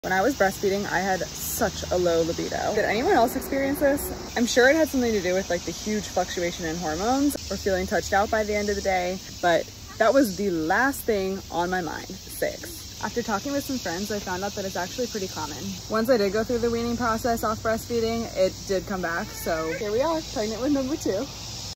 How did you feel about sex when you were breastfeeding?
We've partnered with expert Hannah Witton to help tackle stigma, as well as the physical and psychological challenges of navigating sex as a new mom. The full video is available on our channel.